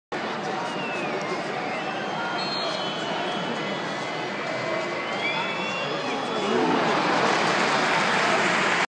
Punizione Di Natale (Udinese - Inter)
Toto Di Natale ci prova con una punizione che sfiora l incrocio dei pali.